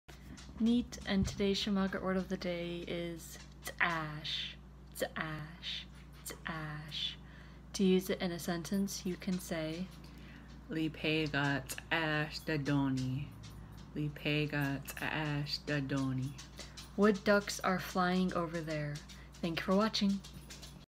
Sm’algyax word for wood duck
Day 28: wood duck

Note: this is also a few other types of ducks, but I don’t have what they are in front of me right now

Tsimshian language